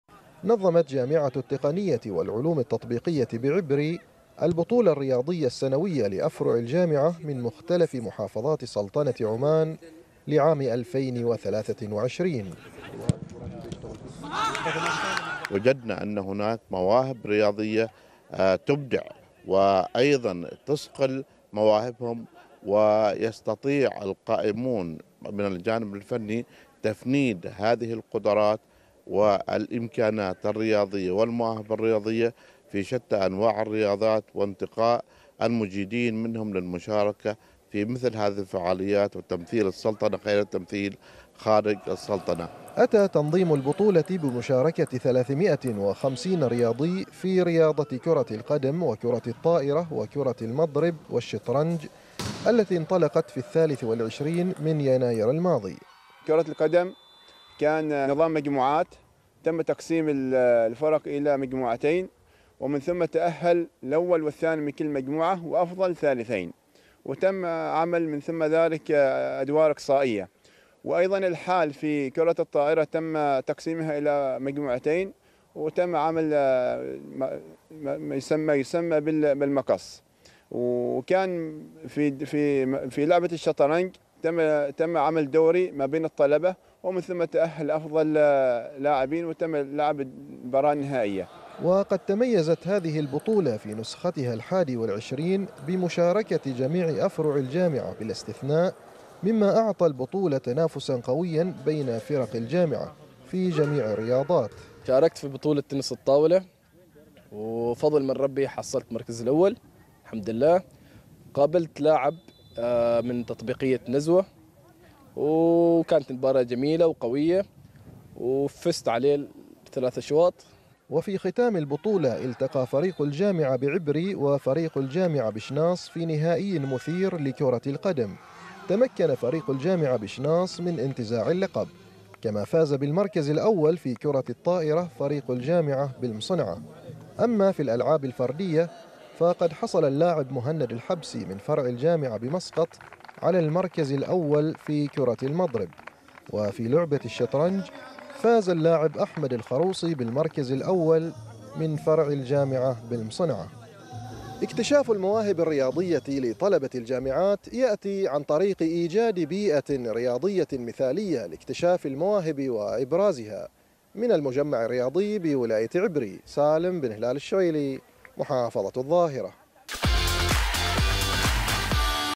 جامعة التقنية والعلوم التطبيقية تختتم البطولة الرياضية السنوية
تقرير : سالم الشعيلي